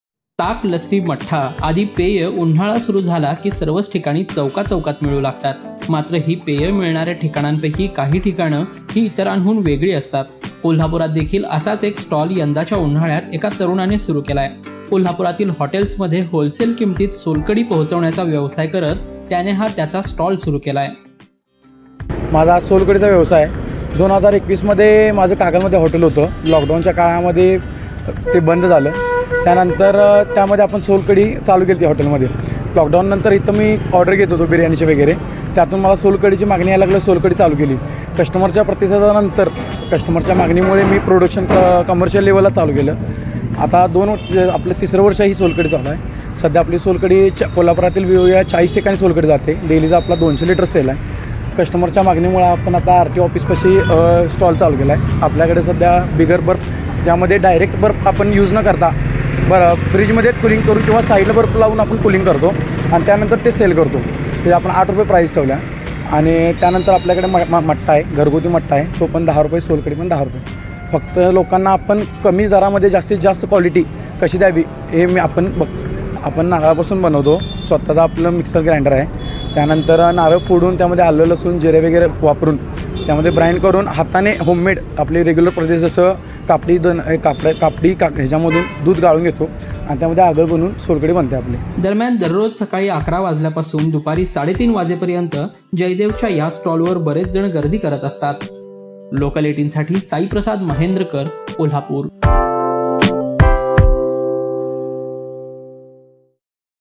Kolhapur Food ताक अन् सोलकढी पिण्यासाठी लागतात रांगा, कोल्हापुरात फेमस आहे हा स्टॉल
ताक, लस्सी, मठ्ठा ही पेयं उन्हाळा सुरू झाला की सर्वच ठिकाणी चौकाचौकात मिळू लागतात. मात्र ही पेयं मिळणाऱ्या ठिकाणांपैकी काही ठिकाणं ही इतरांहून वेगळी असतात. कोल्हापुरात देखील असाच एक स्टॉल यंदाच्या उन्हाळ्यात एका तरुणाने सुरू केला आहे. कोल्हापुरातील हॉटेल्समध्ये होलसेल किमतीत सोलकढी पोहोचविण्याचा व्यवसाय करत त्याने हा स्टॉल सुरू केला आहे.